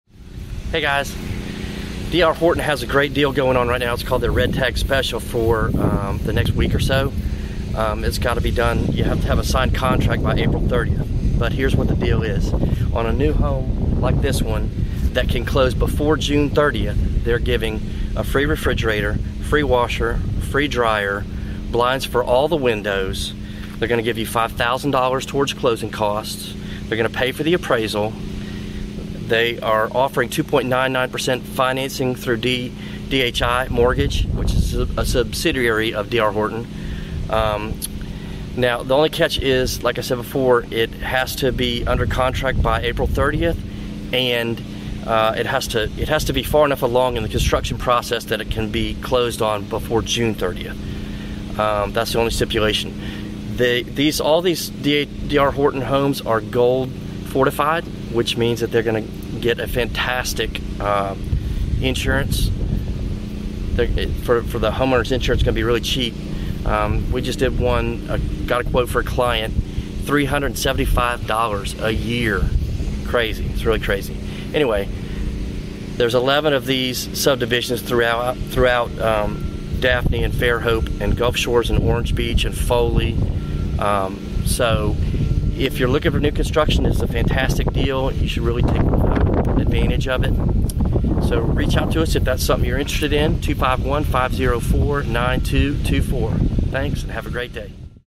Limited Time Only!! DR Horton Red Tag Sale in Baldwin County, Alabama!
Great new construction opportunities in Baldwin County, AL.  Incentive includes $5,000 contribution to buyer's closing costs PLUS a move in package.
* Refrigerator
*Washer and Dryer
*Blinds
*Smart Home features
*Paid Appraisal
*Great financing terms
Home must be under contract by April 30 and close by June 30.

Alison and Greg Foote
CENTURY 21 J Carter and Company